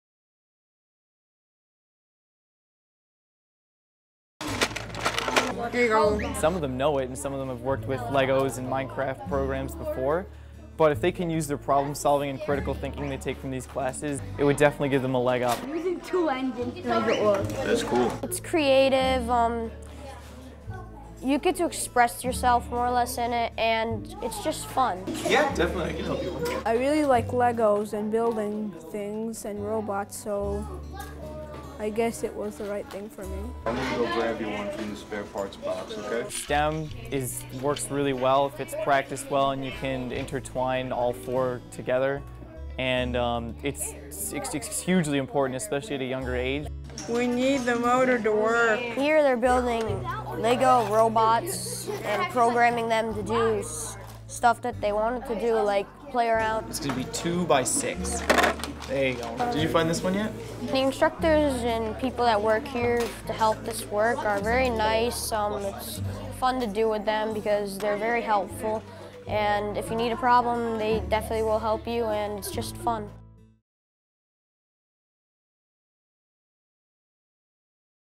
SUNY Oswego's Engaging Creative Minds program
SUNY Oswego's Engaging Creative Minds youth program gives participants a hands-on experience learning about popular and engaging technologies and games like Robotics, Minecraft and developing your own video game. Students engage in a social and collaborative environment that encourages creative thinking and team problem-solving. Participants actively use their imagination to develop innovations of their own and, most importantly, have fun!